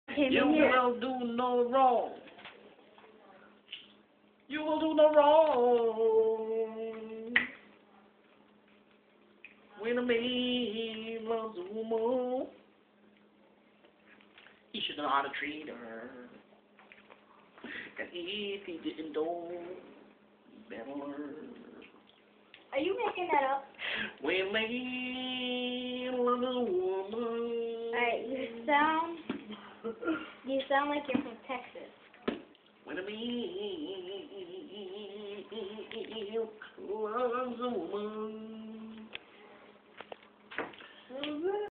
Clyde Watson 423 singing
This is my husband singing like he can sing. he need to get some lessons. ASAP ! clyde watson